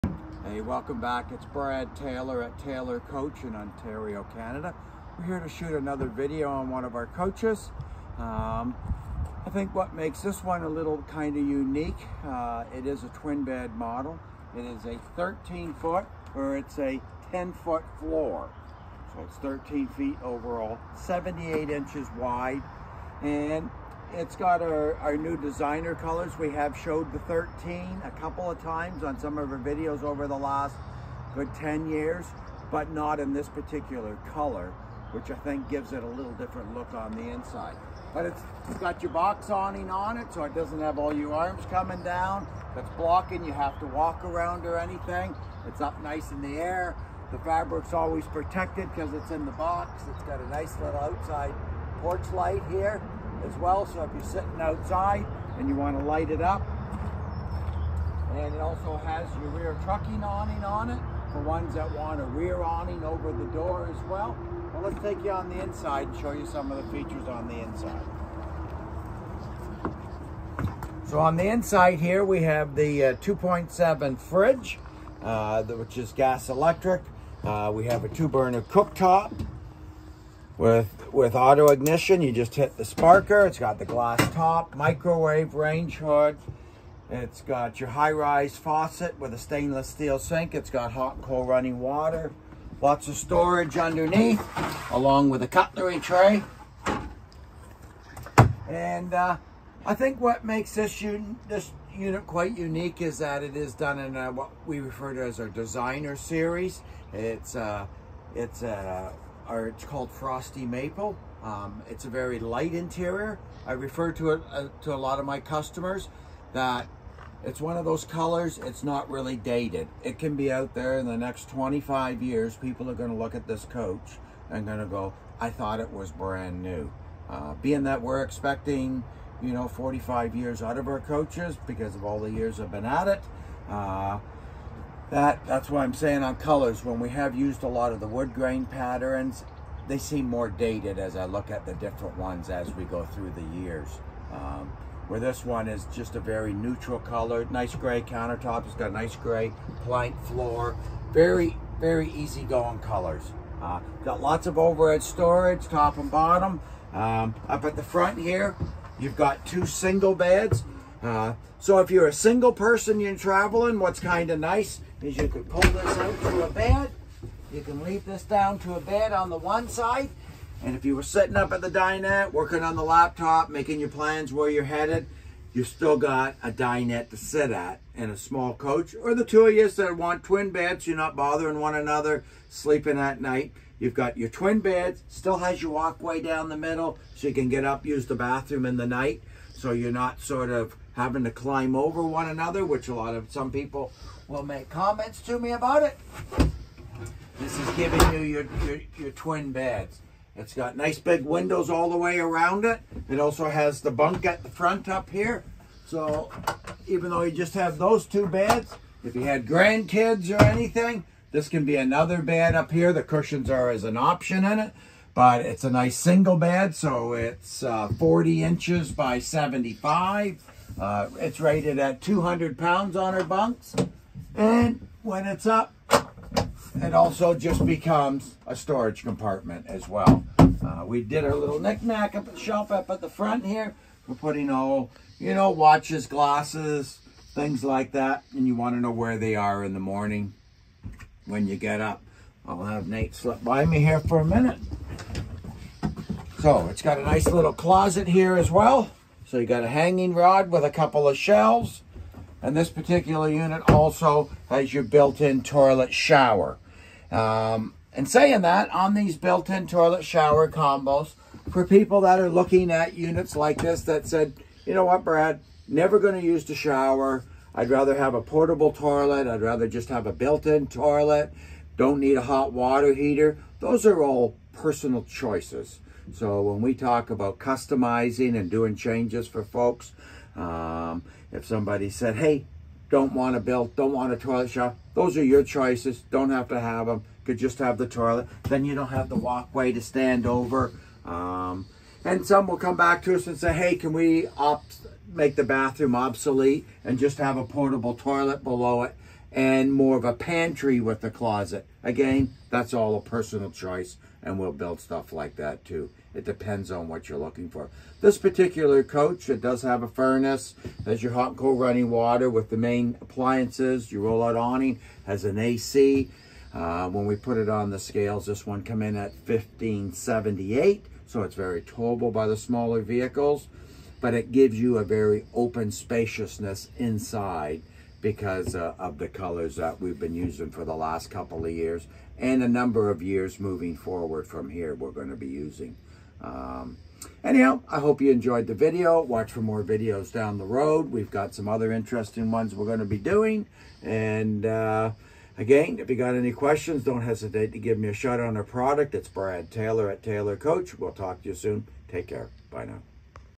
Taylor Coach 2022 13’ twin bed
Here is Brad Taylor showing you a 2022 13’ twin bed. This unit was custom ordered to fill the needs of the owner with 56 year’s experience and quality. The bed size are 32” x 75” gaucho, 30” x 75” dinette and a 40” x 75” bunk.